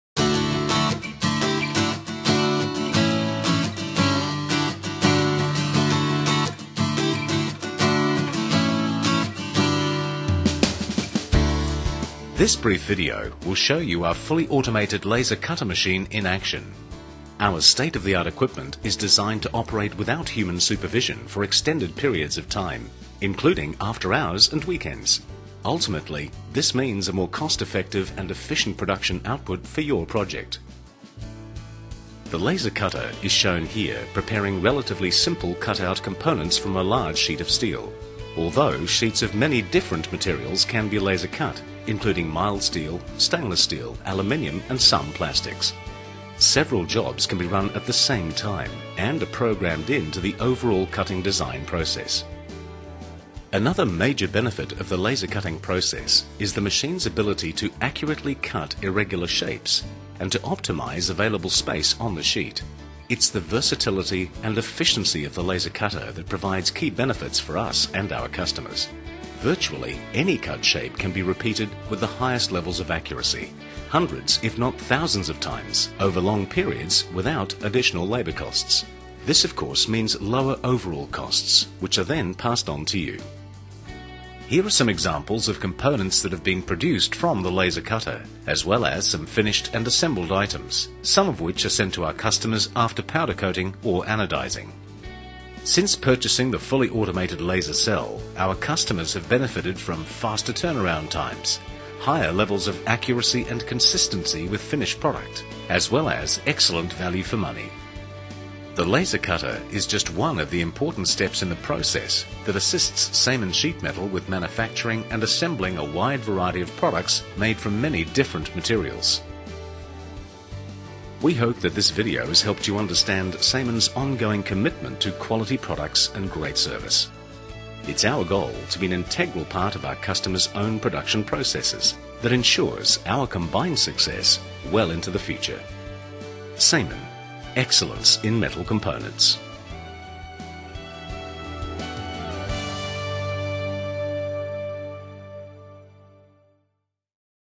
SAMIN Sheet Metal Laser Cutter
This brief video will show you Samin Sheet Metal's fully automated laser cutter machine in action. Our state of the art equipment is designed to operate without human supervision for extended periods of time including after hours and weekends. Ultimately, this means a more cost effective and efficient production output for your project!

Many different materials can be laser cut including Mild steel, Stainless Steel, Aluminium and some Plastic's.

Several jobs can run at the same time and are programmed in to the overall cutting design process.

Another major benefit of the laser cutting process is the machines' ability to accurately cut irregular shapes and to optimise available space on the sheet. It's the versatility and efficiency of the laser cutter that provides key benefits for us and our customers. Virtually any cut shape can be repeated with the highest levels of accuracy - hundreds if not thousands of times, over long periods without additional labour costs. This of course means lower overall costs which are then passed on to you.